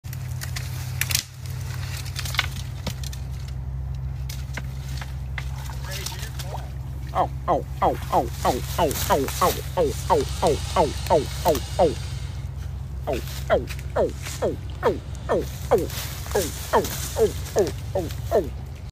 Alligator emerges out of swamp waters and hisses at Florida man
An alligator emerges out of a swamp in Gainesville, Florida. The city is known for having the most alligators in the United States.